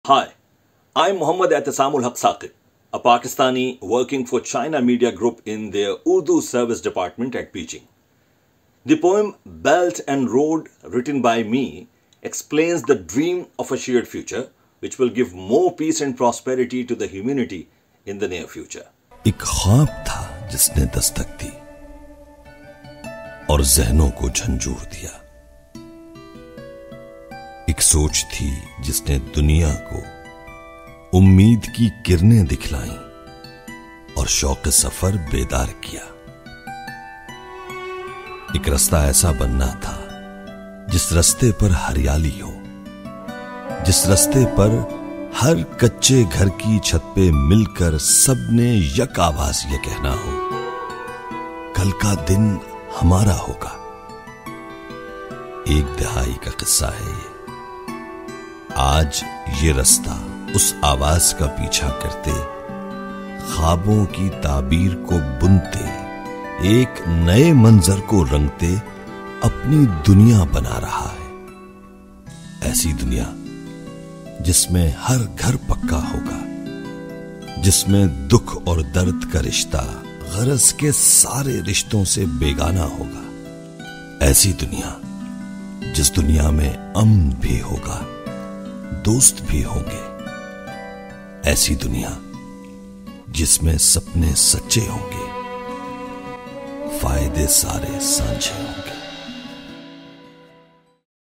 Read a Poem: Imagining the future world in poetry
Muhammad Eatisam Al Haque from Pakistan wrote a poem "Belt and Road," explaining the dream of a shared future which will give more peace and prosperity to the humanity in the near future.